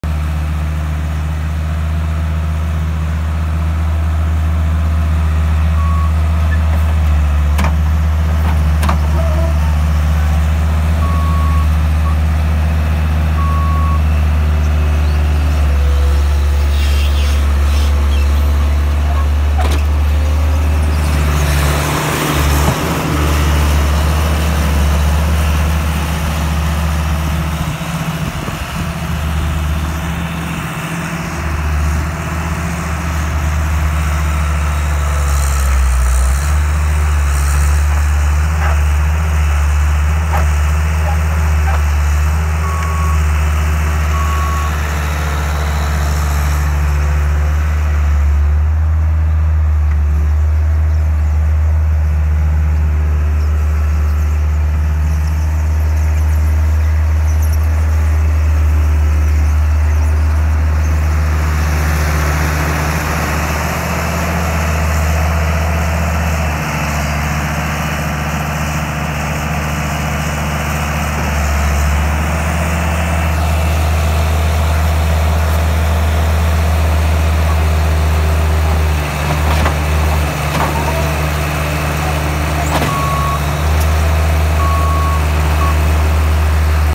Screened Topsoil For Sale in Guelph Ontario loading up triaxle dump truck
Screened Topsoil For Sale in Guelph Ontario Canada $18 per yard 🚜

39,000 yards available commercial and residential deliver and pickup can be arranged.